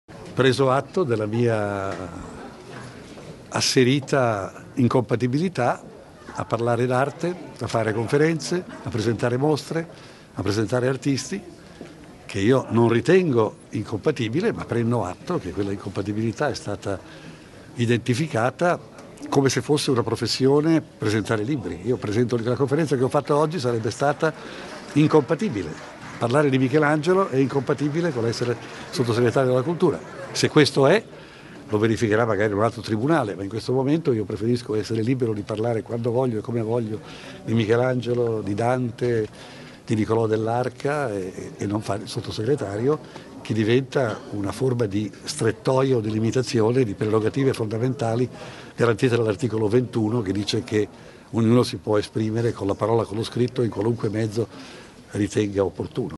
Sgarbi: mi dimetto da sottosegretario per essere libero di parlare
"Ho preso atto della mia asserita incompatibilità"